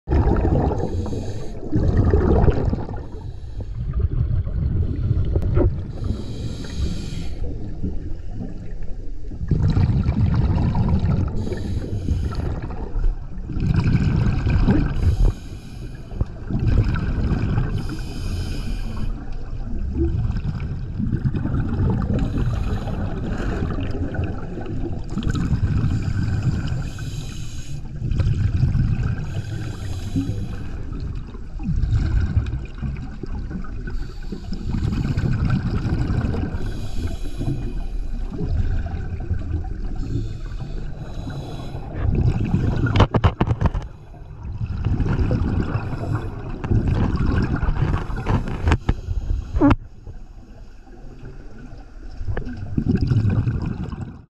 Castle Rocks Aberystwyth June 2022
53 min Dive with Captain Jack from Scubaregs. Visibility was incredible and the footage highlights the extent of beauty in the sea in West Wales.